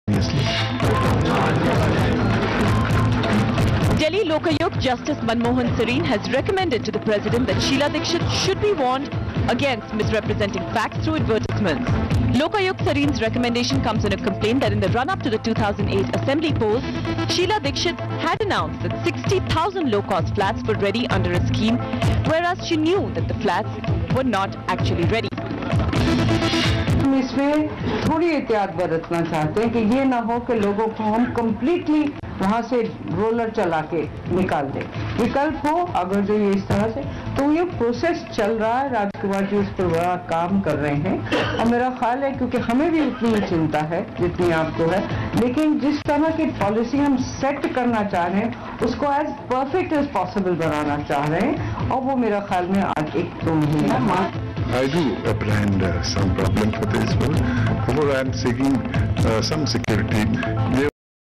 RAJIV RATTAN NDTV24X7JULY190206PM
News clip on the order of the Delhi Lokayukta against Smt Shiela Dikshit in the rajiv rattan awas yojna on the complaint filed by Smt Sunita Bhardwaj, Advocate